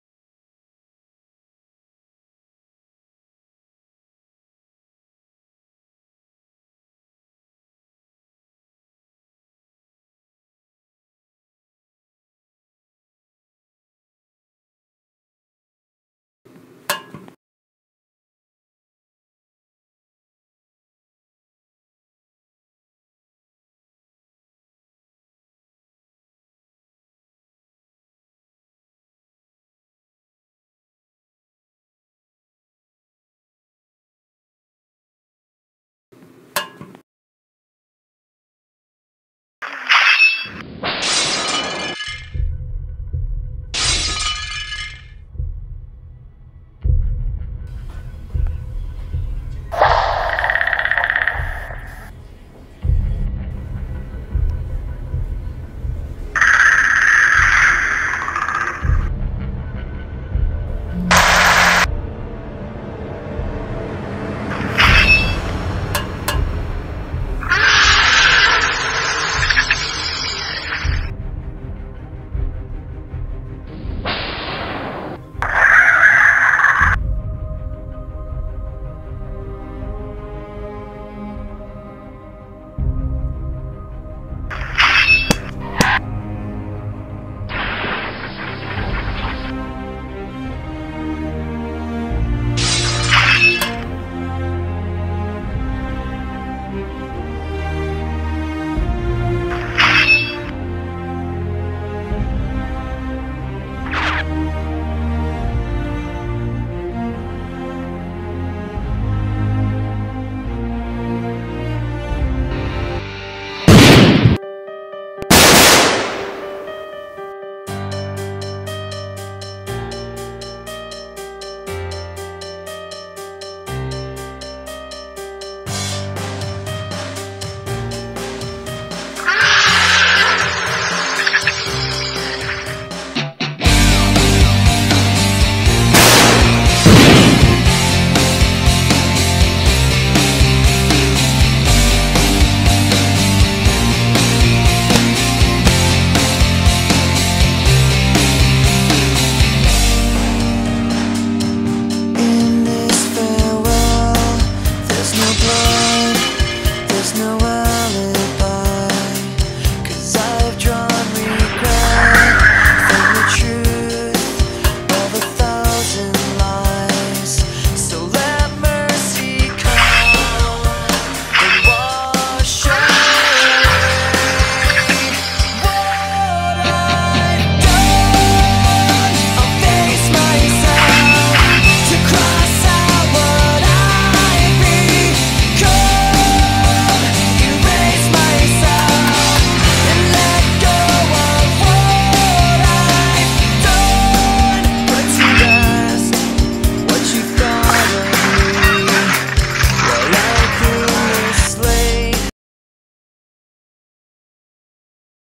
Predator
The mission to kill the sneaky alien monster to save the city